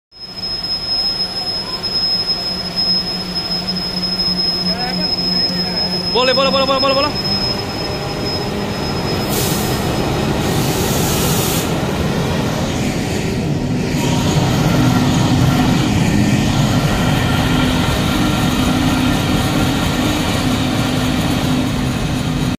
brake binding ke karan rear loco me aag lagi 🔥🔥
*Div- DNR*
*Brake Binding in Loco*
 Heavy brake binding and flame emission from rear engine (23734/NKJ) of EC/DDU/23734/23126, LD 57 BCNHLE + MT at RPR while train entering on up Main line, track also burn at km 628/23-24 as  reported by SM/RPR as well as TI/BXR at 23.25 hrs. Train detaning  at RPR up main line since 23.20 and F/M clear at 23.45 hrs